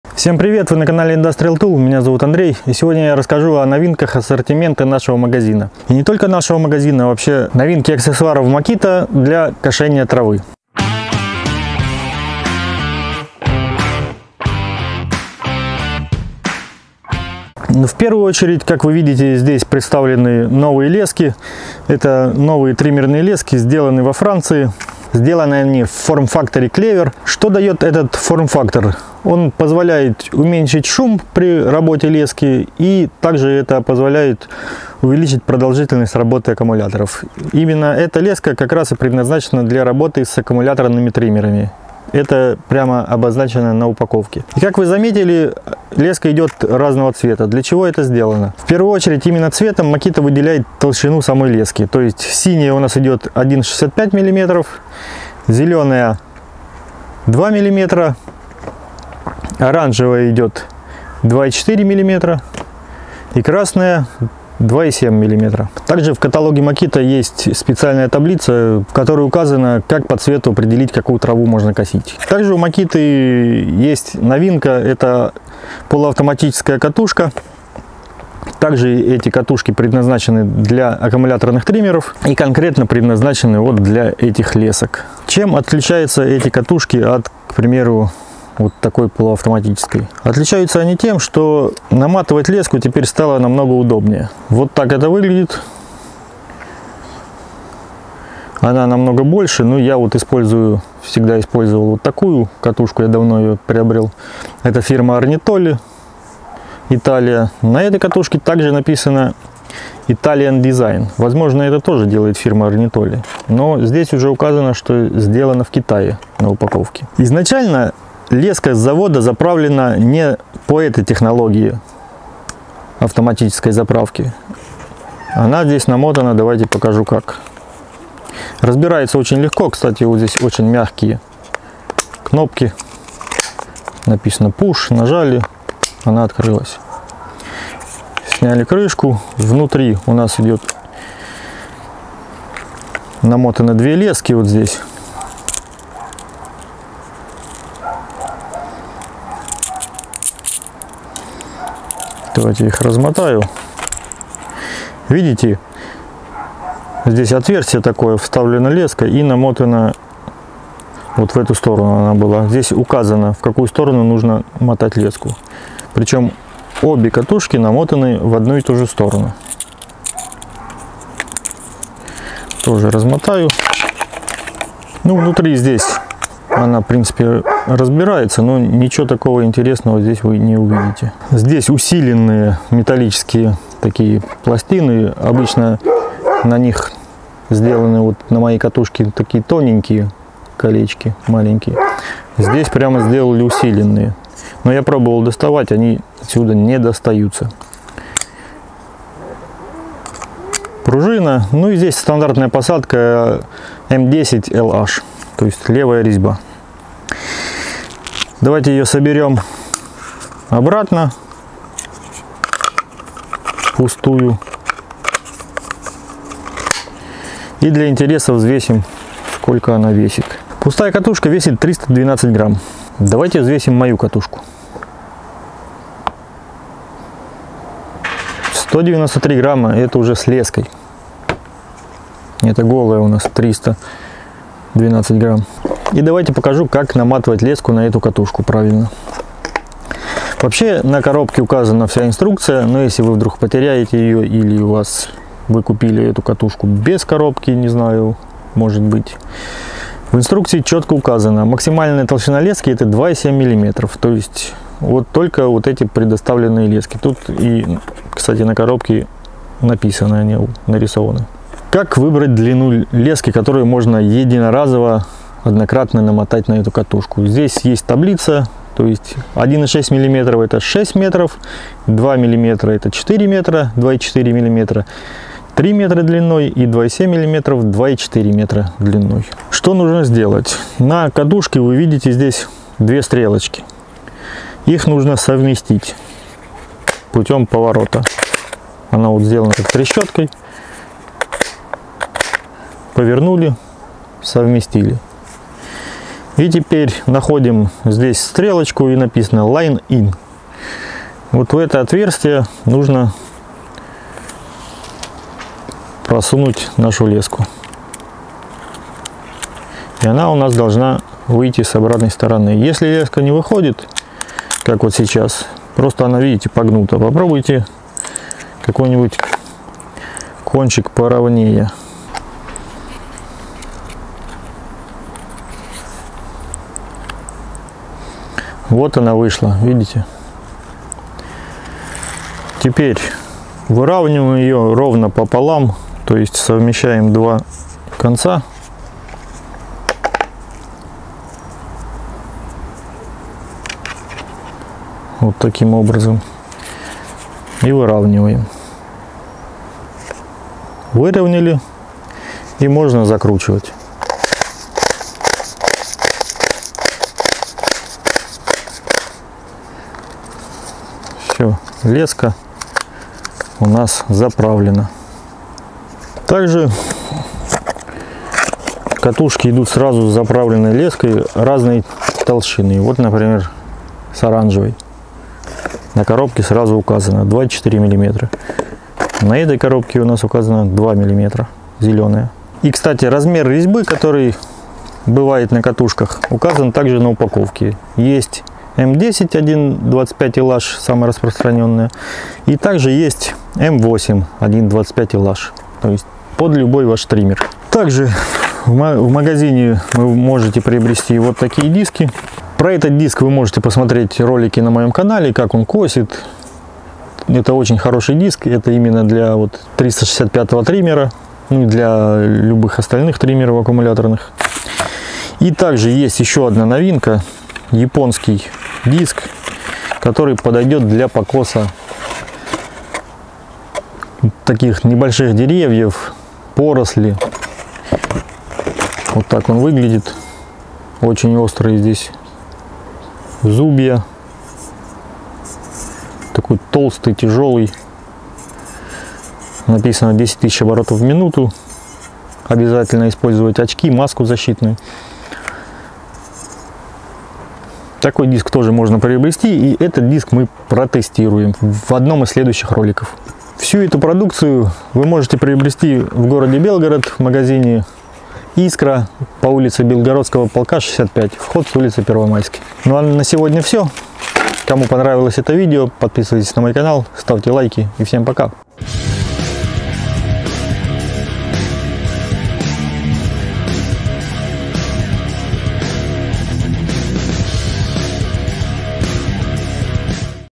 Новая оснастка для триммеров от Makita
В ролике рассказываю о новинках 2021 года для аккумуляторных триммеров.
☝ Триммерная головка полуавтоматическая, М10х1,25LH, диаметр лески 2,4мм быстрая намотка Makita 191D90-9
☝ Триммерная головка полуавтоматическая диаметр лески 2 мм, М10x1.25LH, быстрая намотка Makita 191D89-4
☝ Триммерная головка полуавтоматическая M8x1.25LH, диаметр лески 2 мм быстрая намотка Makita 191D91-7